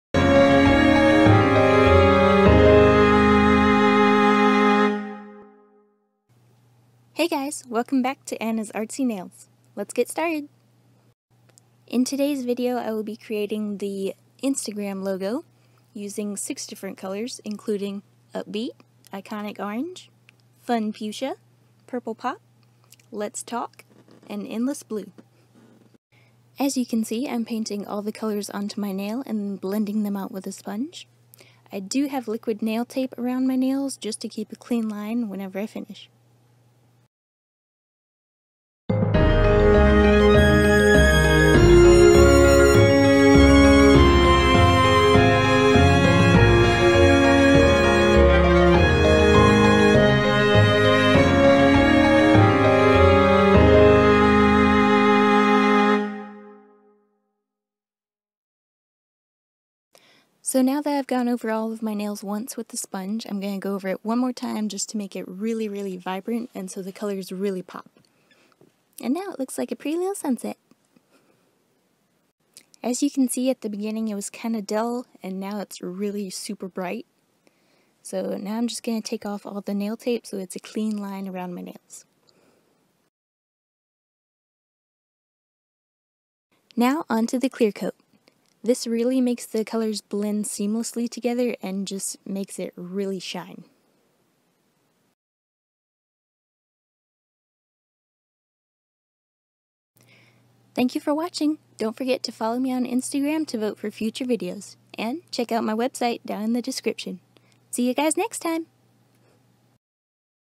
Instagram Worthy Nails!
Nail polish colors: Yellow (Upbeat), Orange (Iconic Orange), Pink (Fun Fuchsia), Purple (Purple Pop!), Violet (Let's Talk), Blue (Endless Blue)
Watch my Instagram story to vote on upcoming nail videos, and check out my posts and my website to see some amazing artwork!

Special thanks to Troy Jarrett for background music